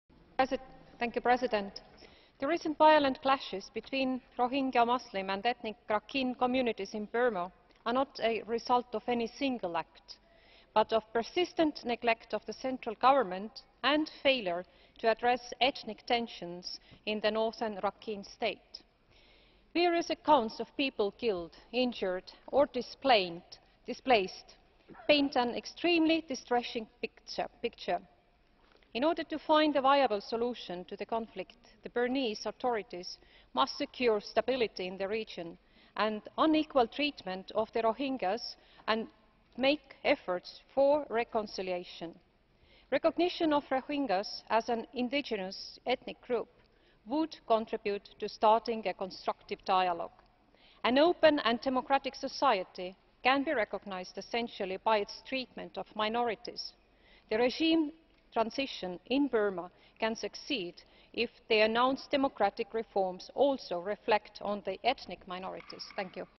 Kristiina Ojuland on Persecution of Rohingya Muslims in Burma
Speech by Kristiina Ojuland MEP (ALDE ADLE) on :
Persecution of Rohingya Muslims in Burma

Ojuland-OR_120913_154444_mesp_en